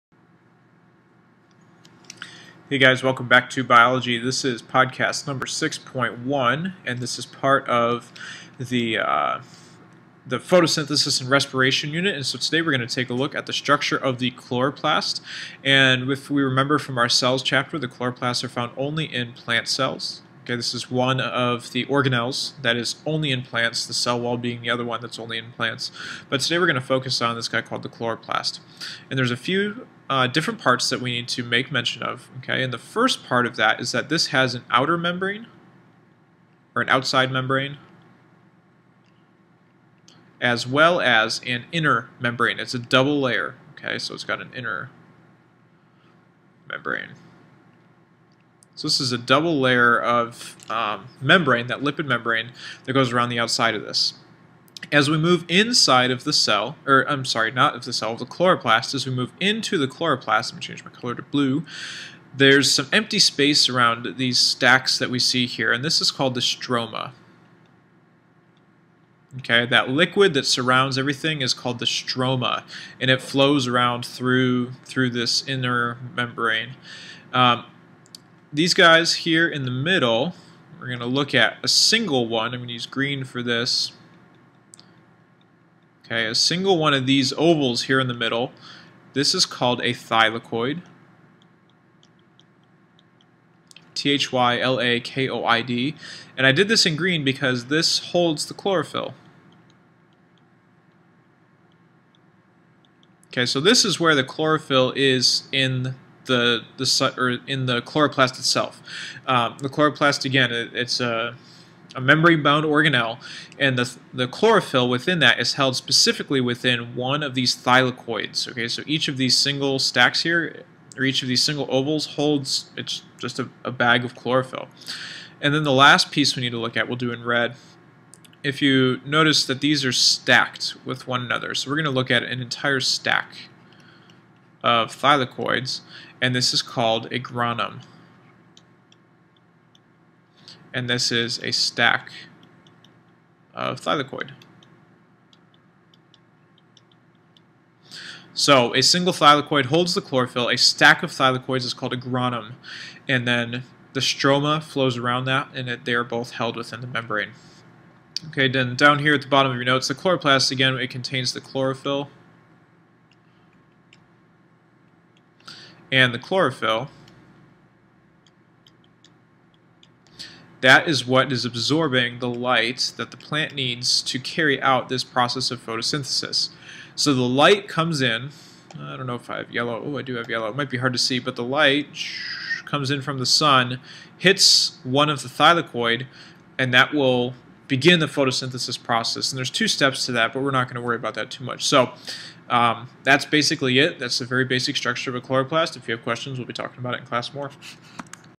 6.1 Chloroplast
A podcast exploring the basic internal structure of the chloroplast.